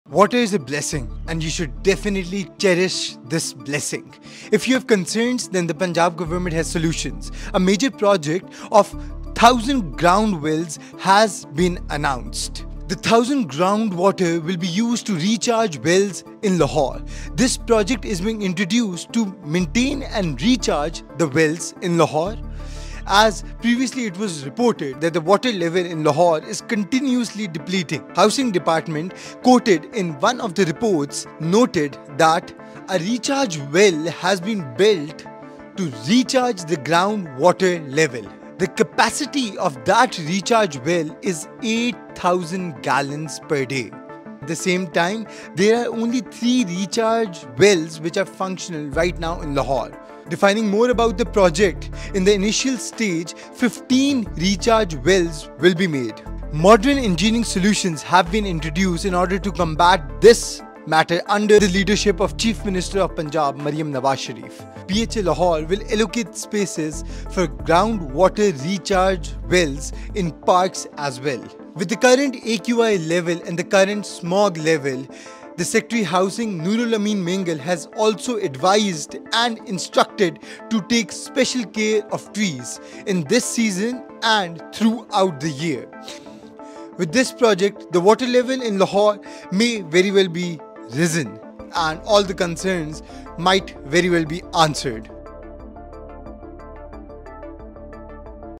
Groundwater Project in Lahore | Abdullah Hammad | Pakistan Connect
A major plan has been prepared to build 1,000 groundwater recharge wells in Lahore to maintain and replenish groundwater levels. According to Housing Department sources, a recharge well has already been built with a capacity of 8,000 gallons per day, while currently only three groundwater recharge wells are functional in the city.

In the initial stage, a total of 15 groundwater wells will be constructed in Lahore. Modern engineering solutions are being implemented under the instructions of the Chief Minister of Punjab. PHA Lahore will allocate space for recharge wells in all parks.

Secretary Housing Noorul Amin Mengal said special care would be taken to protect existing trees during construction. He added that important decisions have been made to prevent environmental pollution. Furthermore, groundwater recharge points will be built across the province under WASA Punjab. He emphasized that water is a blessing and must be saved, and all institutions will ensure the project is completed diligently.